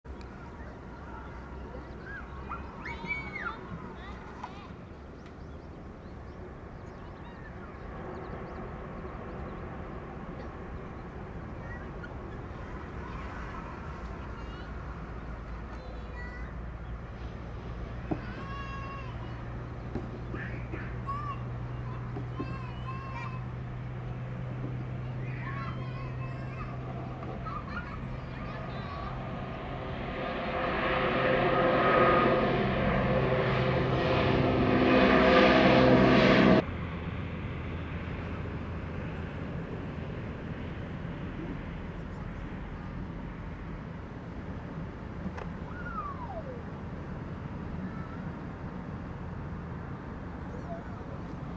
【航空機】高知空港 離陸 De Havilland Canada Dash 8-400 Future Promise Prop(JA461A) Takeoff from KOCHI airport
2024/03/31 AKX1612 伊丹行

Camera：Nikon Z50&FTZ&AF-S NIKKOR 80-400mm f/4.5-5.6G ED VR